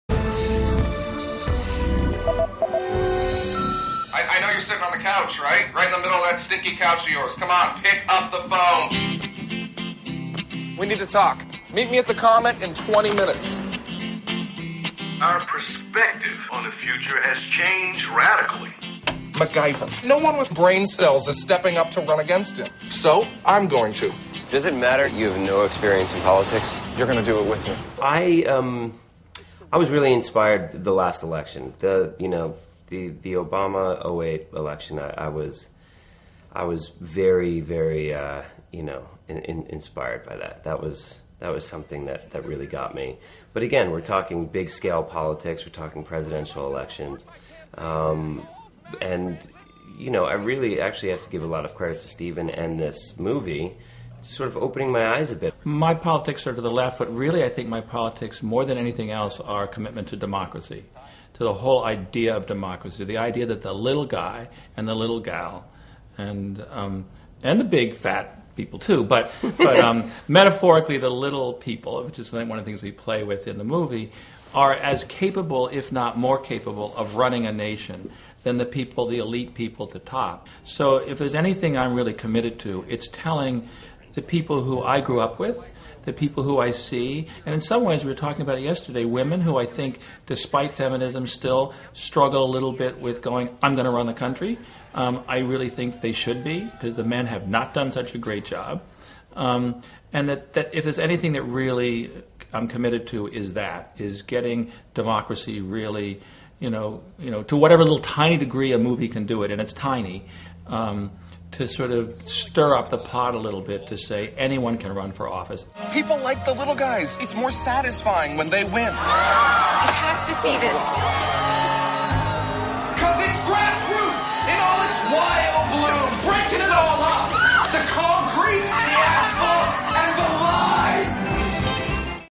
Jason Biggs Tackles 'Grassroots' Politics
Jason Biggs discusses his revelations about politics after starring in dramedy 'Grassroots,' while director Stephen Gyllenhaal explains the idea behind the film. (Nov. 5)